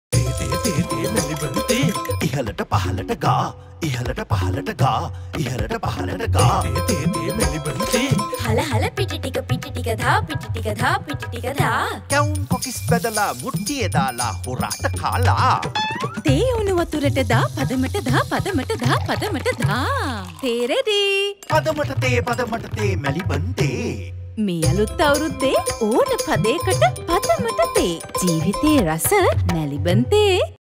Maliban Tea - ඕන පදේකට පදමට තේ
කැවුම් කොකිස් සාදා
මැලිබන් තේ උණු වතුරට දා පදමට දා
පිටි ටික දා සීනිත් දා බොමු අවුරුදු දා.